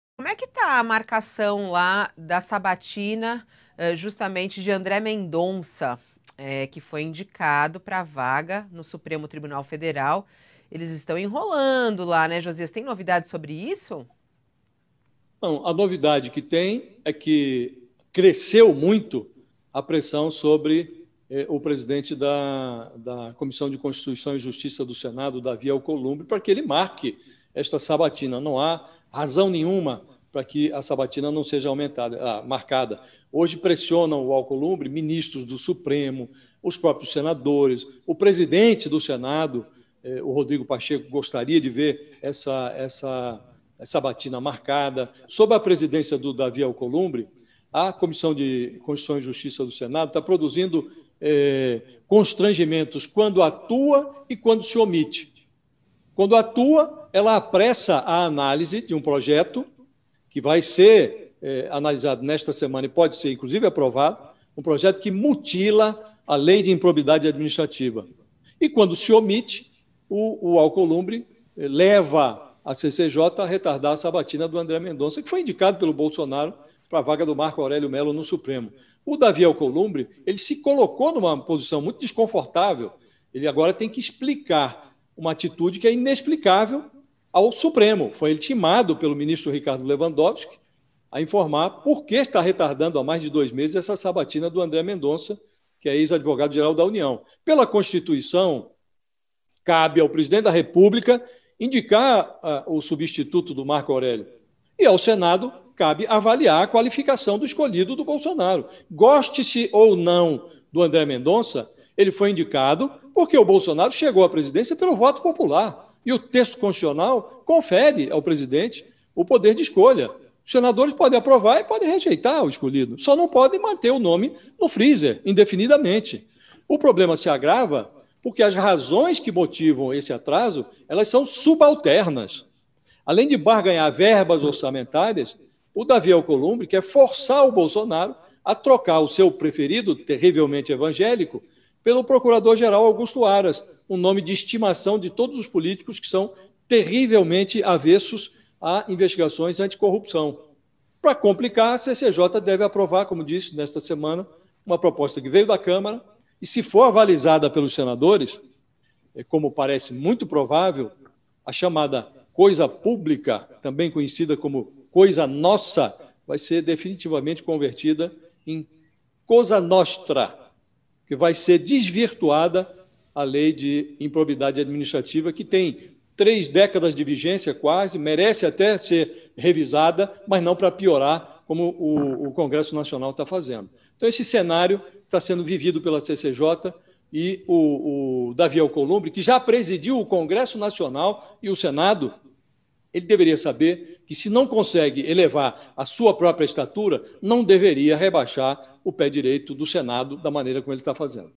Não há razão para que a sabatina de André Mendonça não seja marcada | Josias de Souza
Josias de Souza, colunista de política do UOL, comentou hoje, durante participação no UOL News, sobre o adiamento da sabatina de André Mendonça no Senado. Mendonça foi indicado pelo presidente Jair Bolsonaro (sem partido) para a vaga de ministro do STF e precisa da aprovação dos senadores para ocupar o cargo.